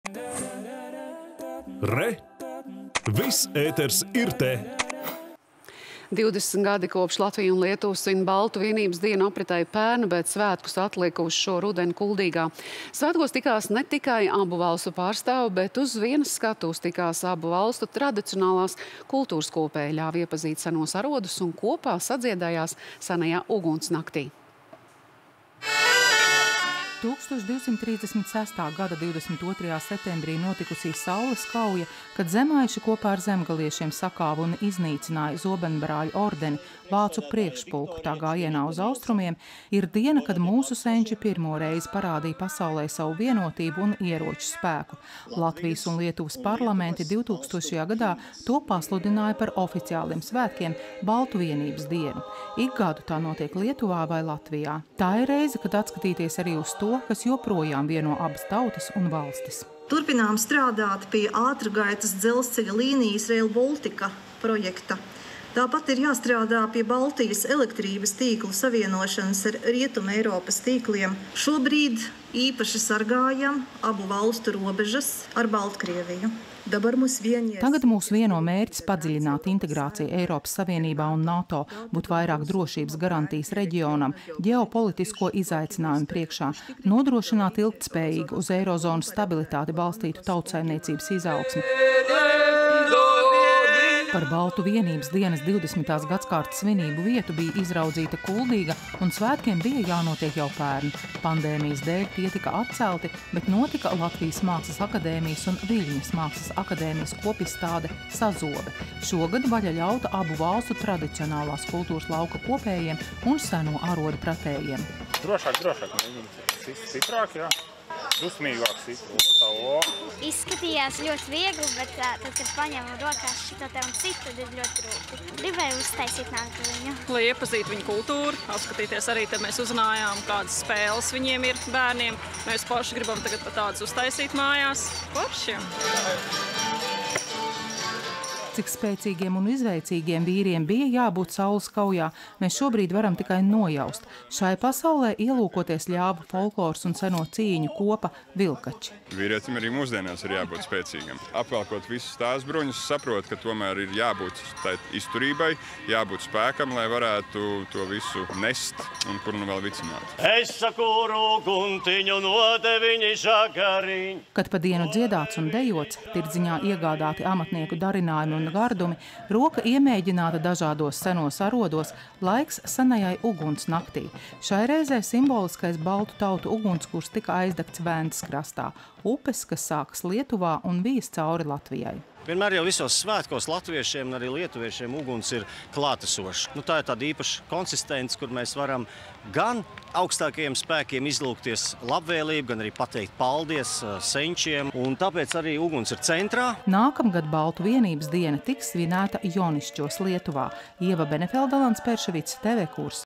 Baltu vienības diena Kuldīgā
20 gadi, kopš Latvija un Lietuva svin Baltu vienības dienu, apritēja pērn, bet pandēmijas dēļ svētki tika atlikti uz šo rudeni. Mājasmātes loma šoreiz bija uzticēta Kuldīgai. Svētkos tikās ne tikai abu valstu pārstāvji, bet uz vienas skatuves tikās abu valstu tradicionālās kultūras kopēji, ļāva iepazīt senos arodus un kopā sadziedājās Senajā uguns naktī.